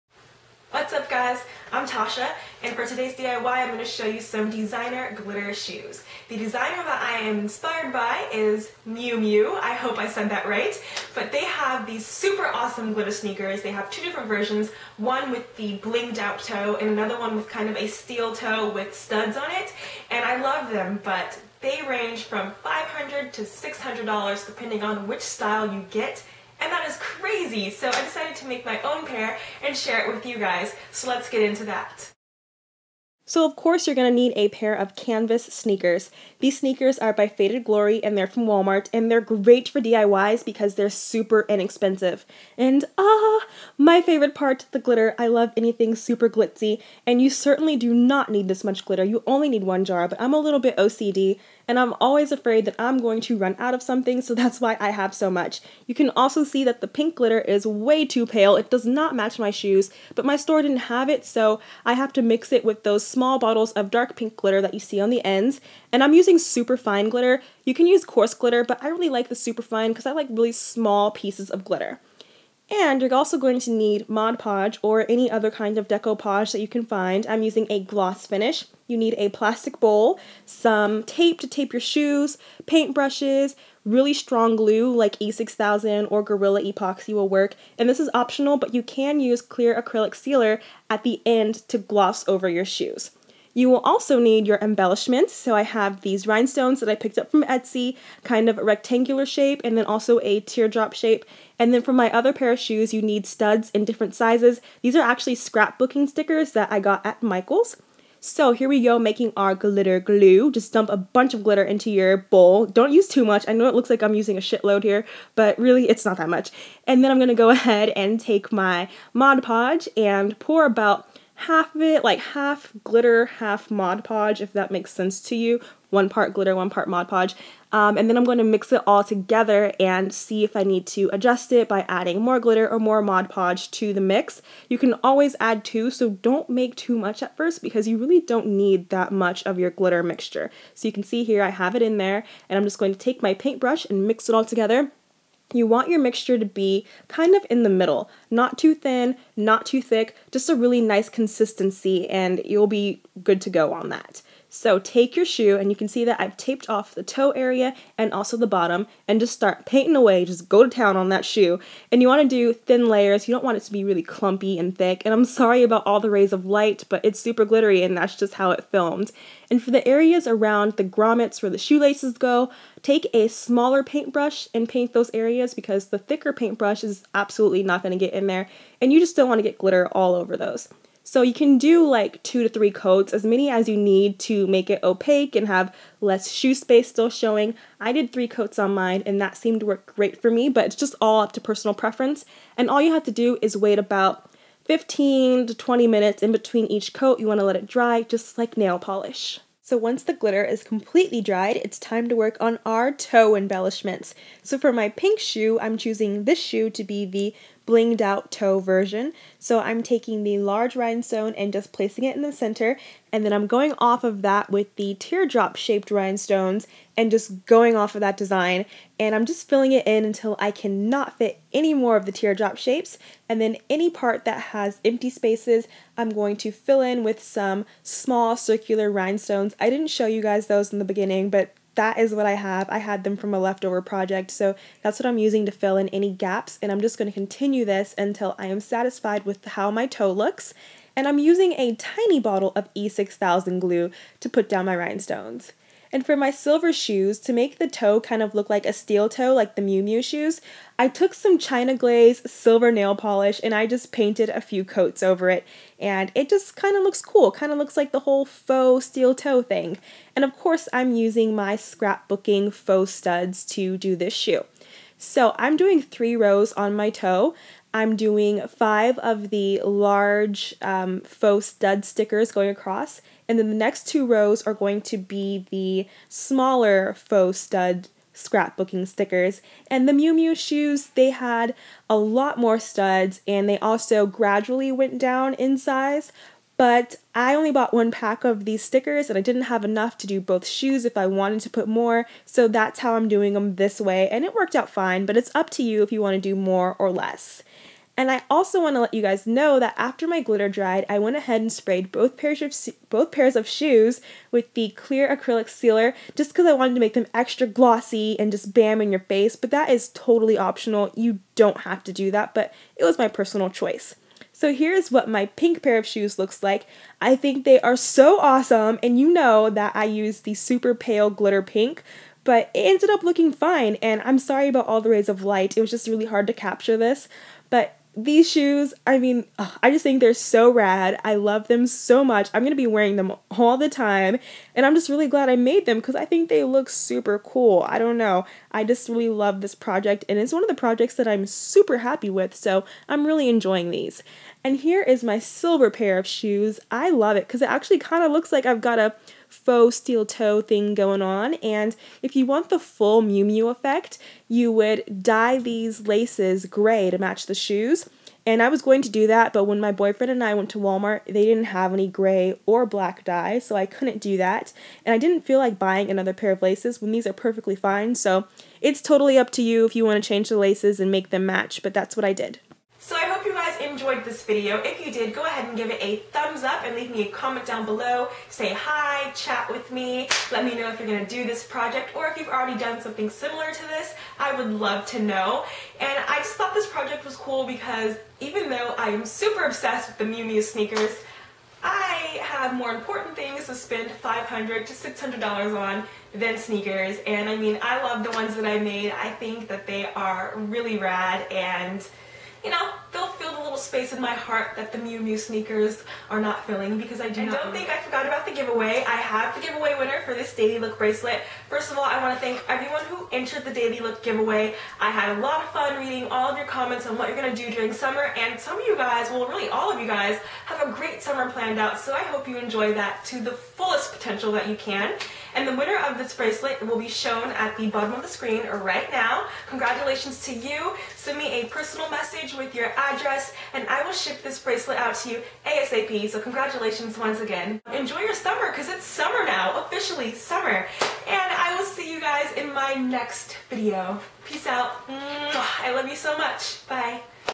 DIY Miu Miu Glitter Sneakers & Giveaway Winner!
Two versions of the Miu Miu glitter sneakers. DIY style!